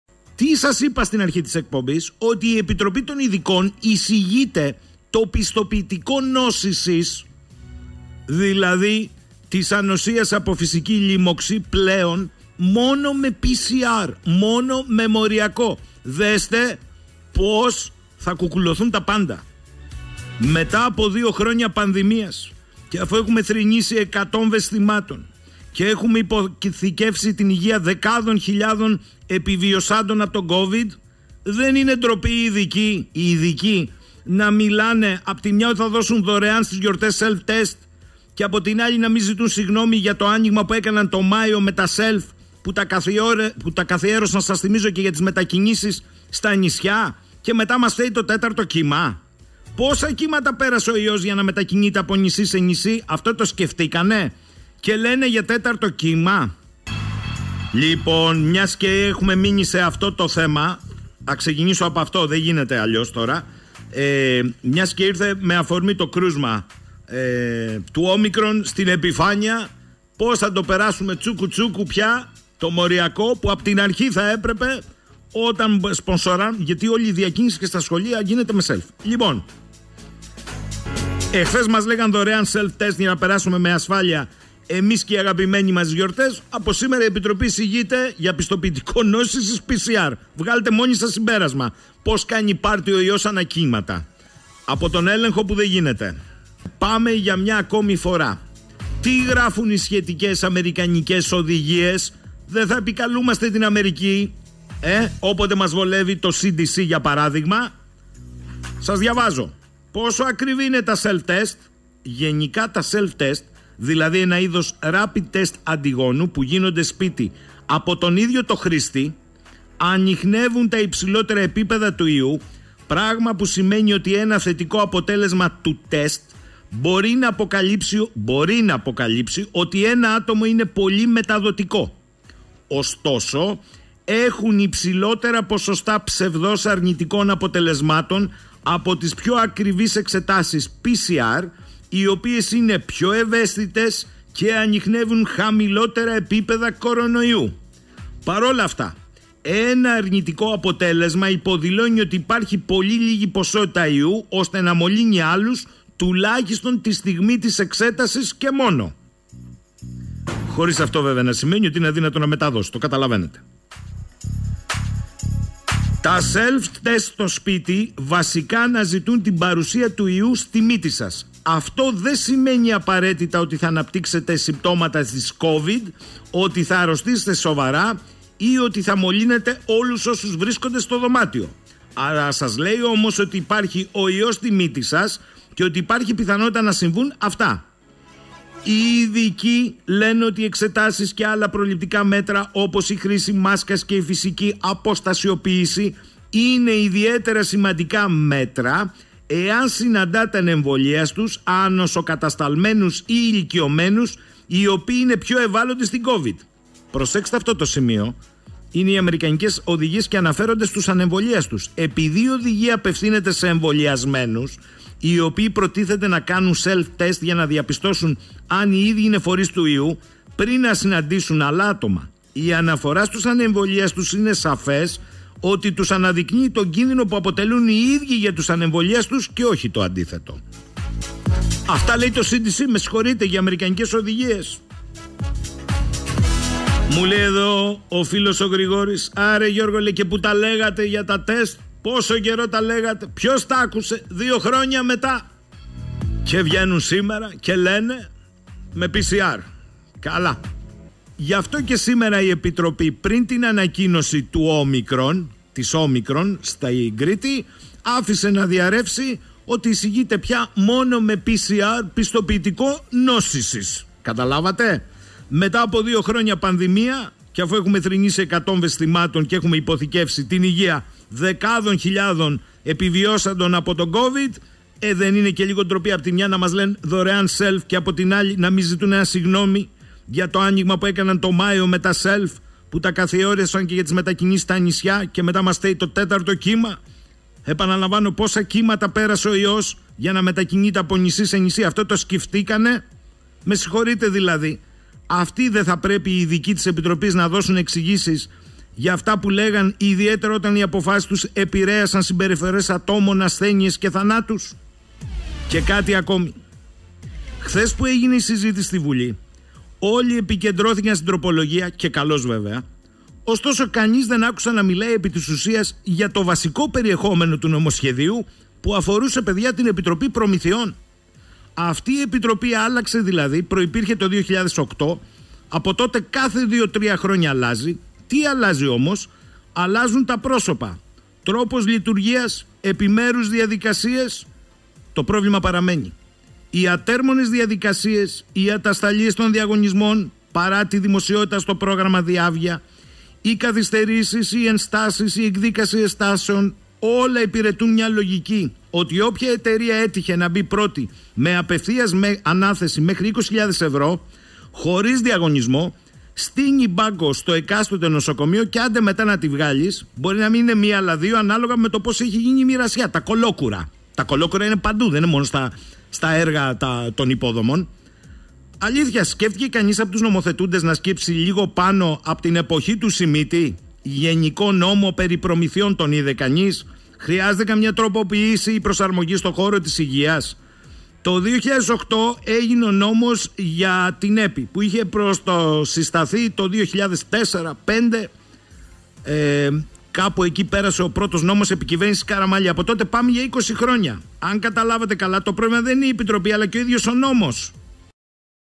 PCR , η «καραμέλα» των self και η «επαλήθευση» της Ο
Στη σημερινή ανάλυση της εκπομπής με τον Γ.Σαχίνη στον 98.4 , παρατέθηκε αναλυτικά η εισήγηση της επιτροπής των ειδικών, πως για τα πιστοποιητικά νόσησης  και άρα ανοσίας από φυσική λοίμωξη, θα πρέπει να απαιτείται το αποτέλεσμα από εξέταση PCR, ως το πλέον αξιόπιστο, την ώρα που ακόμη και σήμερα ο Υπουργός Υγείας , δήλωνε πως ενόψει τον γιορτών, θα διανεμηθούν δωρεάν self tests , για να έχουν επιδημιολογική εικόνα της χώρας !
Η ανάλυση αφού παρέθεσε τι λένε οι αμερικανικές οδηγίες για το self που έχουν λάβει και την έγκριση του CDC, αναρωτήθηκε, αν 2 χρόονια μετά την πανδημία και αφού ε΄χουμε θρηνήσει εκατοντάδες νεκρών και έχουμε υποθηκεύσει την υγεία δεκάδων χιλιάδων επιβιωσάντων από την Covid-19, ειδικοί και πολιτικοί διαχειριστές, έχουν αντίληψη τι σημαίνει να μιλάνε ακόμη για δωρεάν self.
 Άλλωστε το άνοιγμα από τον Μάιο και μετά στα self στηρίχθηκε για τις μετακινήσεις ιδίως στα νησιά την ώρα που ρίχναμε το φταίξιμο έτσι απλά σε ένα 4ο κύμα . Πόσα κύματα, αναρωτήθηκε η εκπομπή πέρασε ο ιός για να μετακινείται από νησί σε νησί ; Πολύ περισσότερο που το πρώτο ταυτοποιημένο κρούσμα Ο που εντοπίστηκε στην Ελλάδα , στα Χανιά, αφενός συνέβη μετά από δύο αρνητικά rapid , αφετέρου είναι το πρώτο ταυτοποιημένο , αλλά άγνωστο αν είναι το μοναδικό της Ο , με την χαμηλή γονιδιωματική επιτήρηση στην Ελλάδα και μία επιδημιολογική που ως φάινεται στηρίζεται στα ανακριβή self .